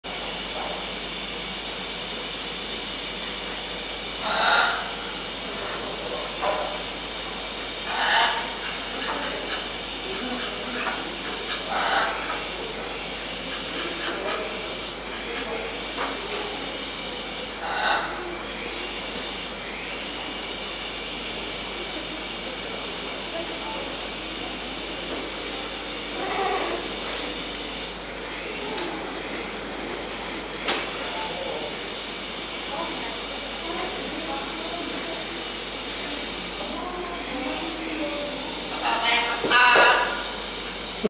4K マロロちゃんお座りしておなかポリポリ♪ フタユビナマケモノ 熱帯の森 2020年3月19日 那須どうぶつ王国にて C0295
マロロちゃんお座りしておなかポリポリ♪
フタユビナマケモノ
熱帯の森
2020年3月 19日